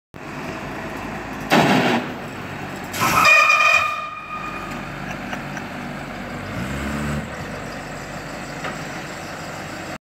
Loading the bus
Had a slight breakdown and had to pick her up in the morning!